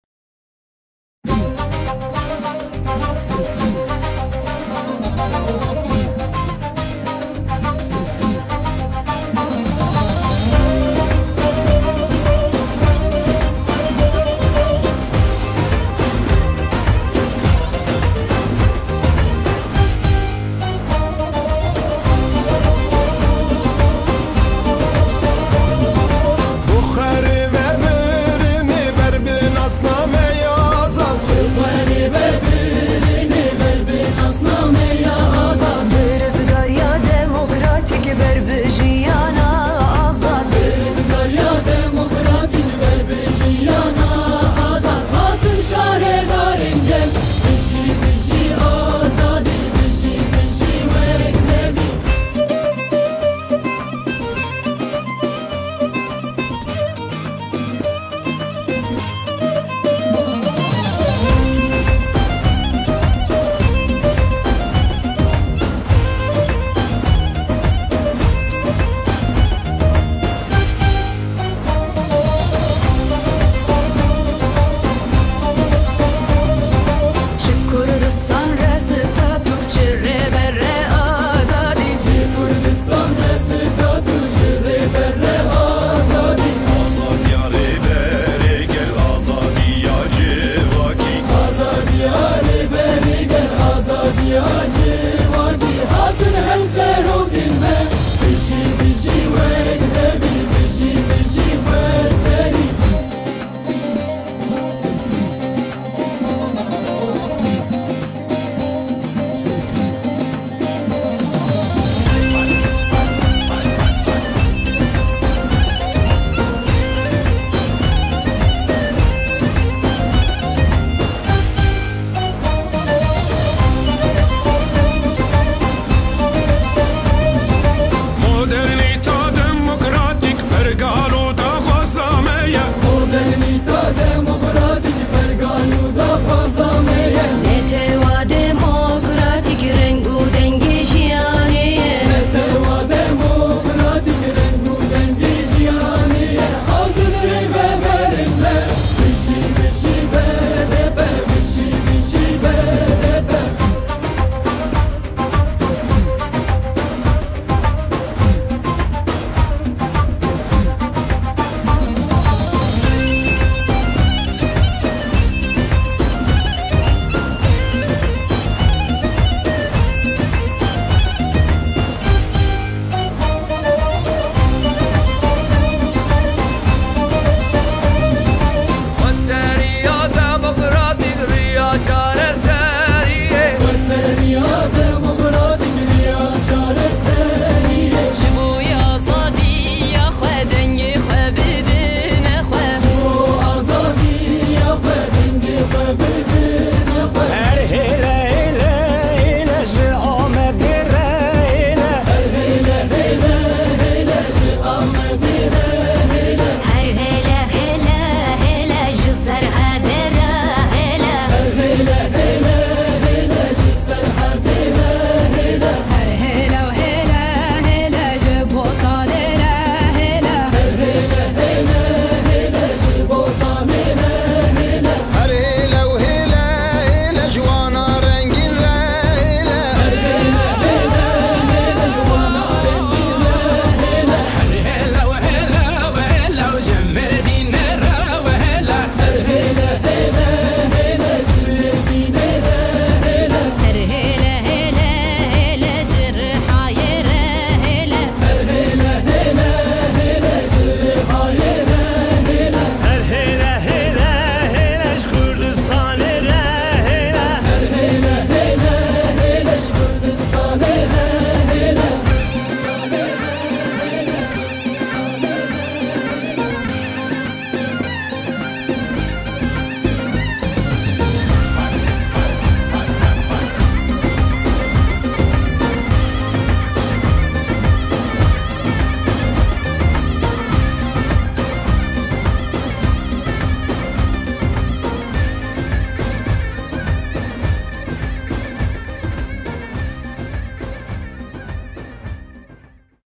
BDP Siirt Eşbaşkanları: Proje Tanıtımı
BDP Siirt Belediyesi Eş Başkan Adayları;
Sn. Tuncer BAKIRHAN ve Sn. Belkız EPÖZDEMİR'in
"Siirt Senin" şiarıyla Halka sunduğu "56 PROJE'...